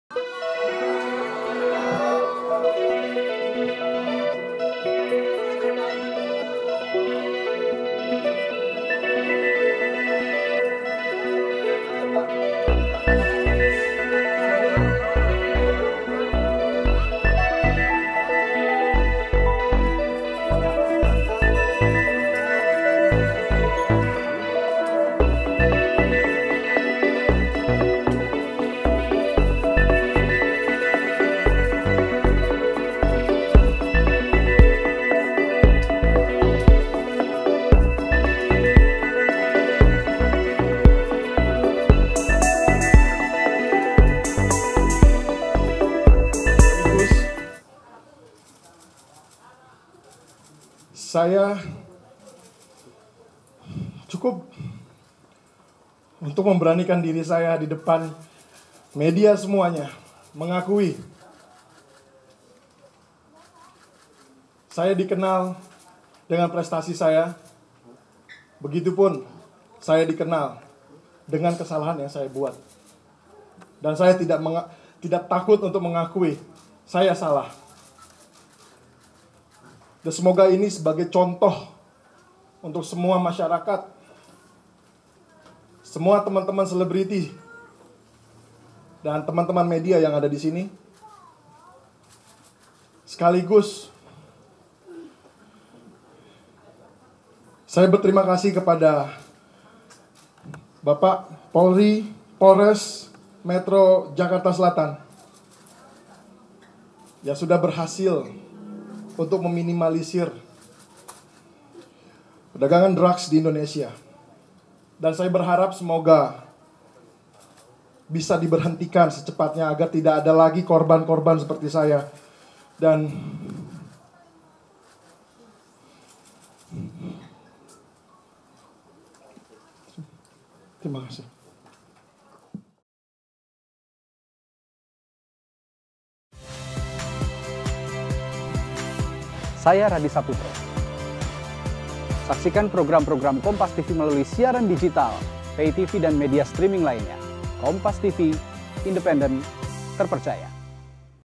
Ditangkap Karena Sabu, Ammar Zoni Harap Peredaran Narkoba di Indonesia Berhenti
JAKARTA, KOMPASTV - Selebritas Ammar Zoni sampaikan harapannya agar peredaran narkoba di Indonesia dapat berhenti.

Hal tersebut diungkapkan saat konferensi pers di Polres Metro Jakarta Selatan, Jumat (10/3).

"Saya berterima kasih pada bapak Polri, Polres Metro Jakarta Selatan yang sudah berhasil untuk meminimalisir perdagangan drugs di Indonesia," ungkap Ammar.

"Saya berharap bisa diberhentikan secepatnya agar tidak ada korban-korban lagi seperti saya," lanjutnya.

Ammar pun tegas mengaku salah dalam kasus ini. Ia ditangkap karena memiliki dan menggunakan narkoba jenis sabu.

Polisi pun amankan sabu dengan berat lebih dari 1 gram dari lokasi penangkapan Ammar Zoni.

"Saya dikenal dengan prestasi saya, begitupun saya dikenal dengan kesalahan yang saya buat. Saya tidak takut mengakui saya salah," ucap Ammar.